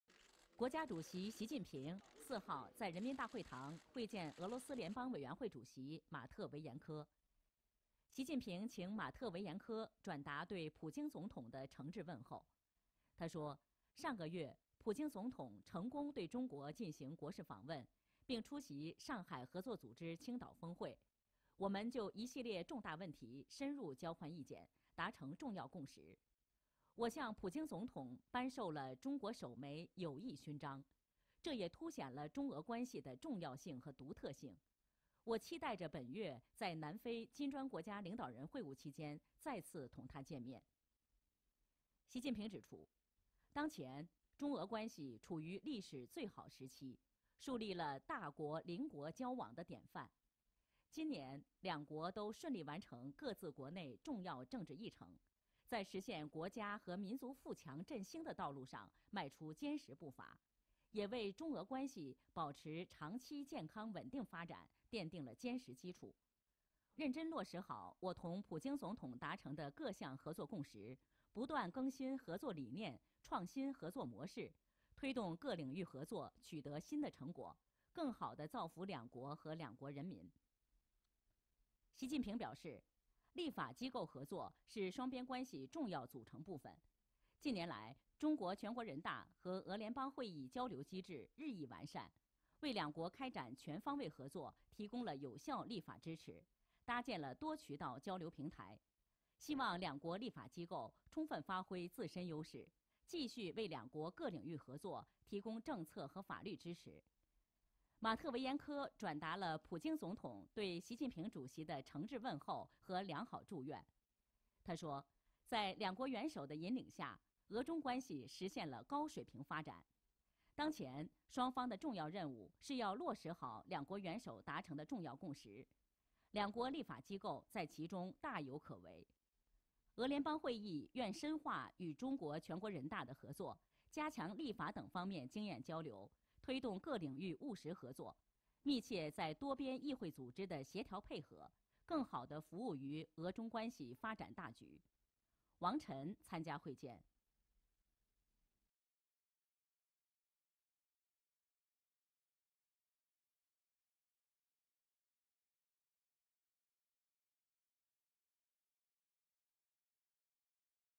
Xi Jinping Meets Russian Federation Council Speaker
Chinese President Xi Jinping met with Russian Federation Council Speaker Valentina Matviyenko at the Great Hall of the People in Beijing on Wednesday.